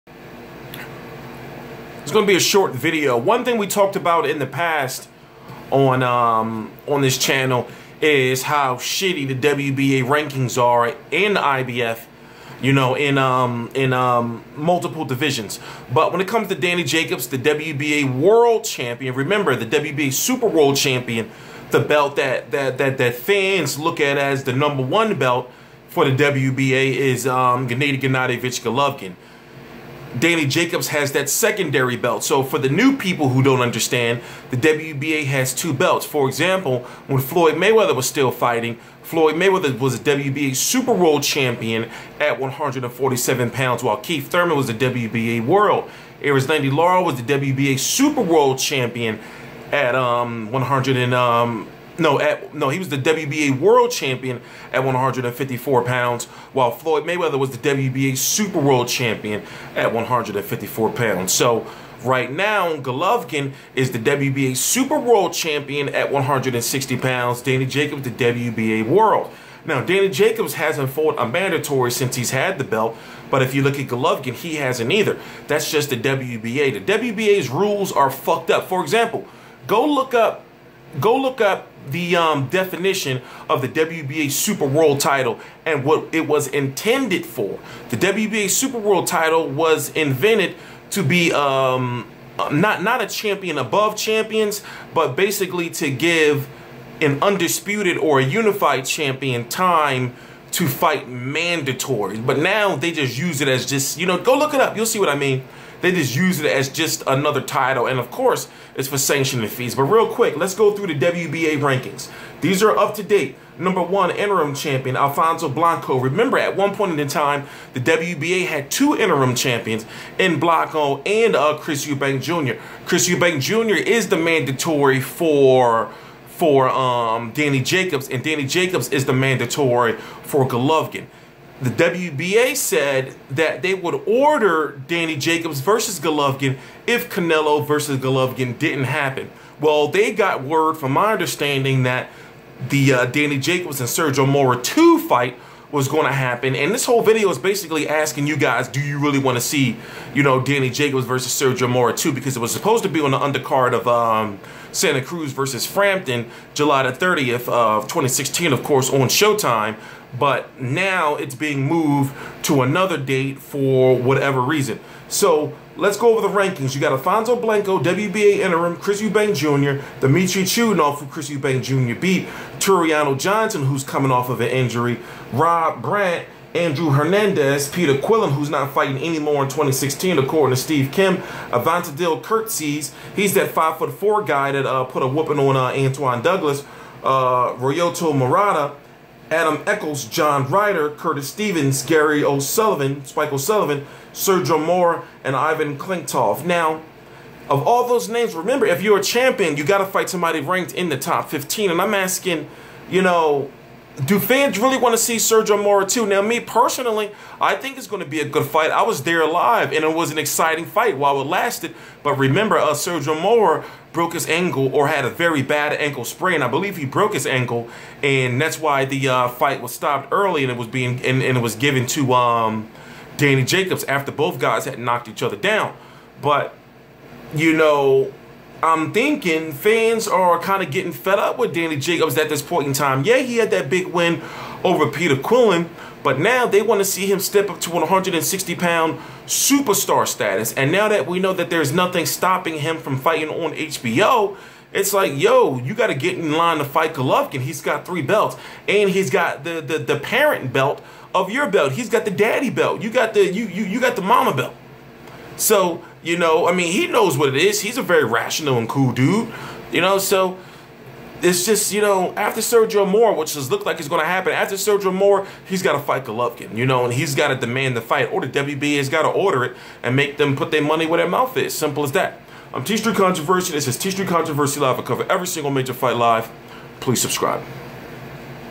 JACOBS VS MORA 2 LIKELY! MOVED FROM 7/30/16! OWN CARD? WBA TO ORDER WINNER TO FACE GOLOVKIN?
- Jacobs vs Mora 2? -